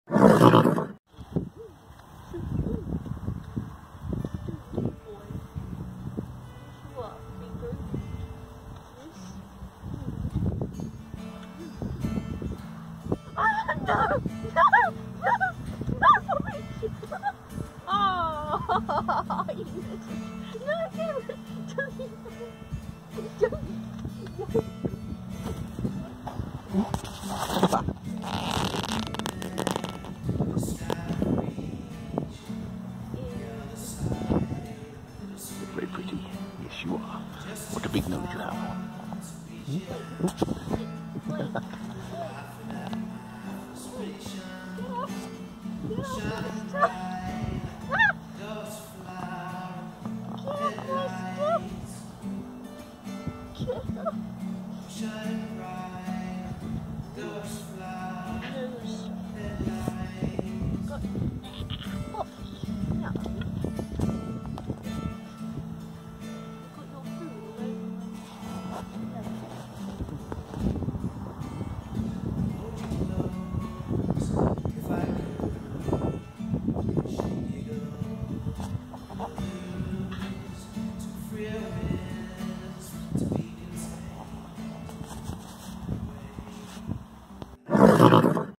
Horse tries to eat monkeys gloves
Bob was getting super hungry & Girl monkey had her gloves loose on her lap.  they must have smelled good cos he tried to eat them!